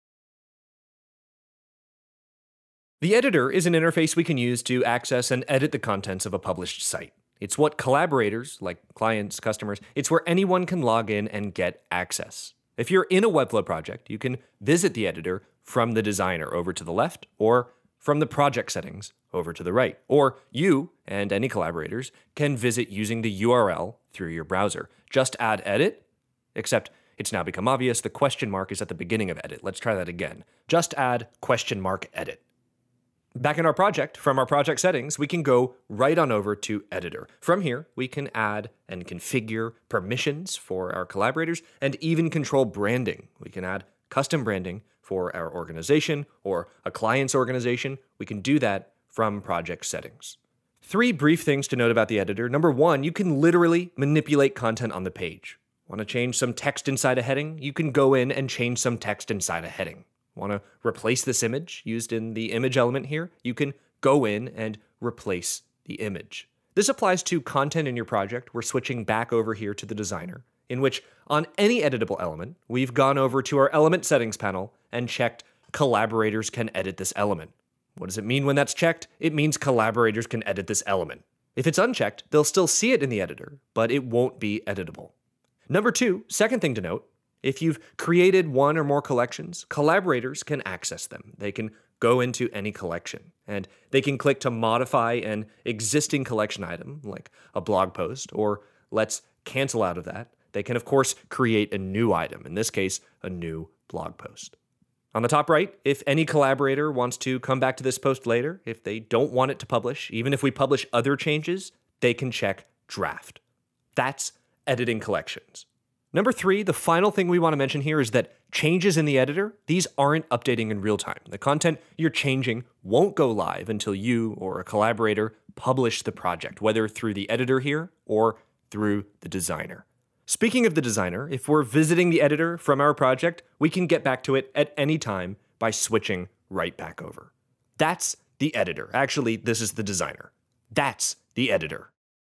Using the Editor for clients and collaborators - Webflow tutorial
The Webflow Editor makes editing the website and publishing new content, really easy for your client or your team. The Webflow Designer is a powerful tool for building websites, whereas the Webflow Editor is a simplified interface that’s focused on editing website content. You can edit content right on the page, or in the panels below the page.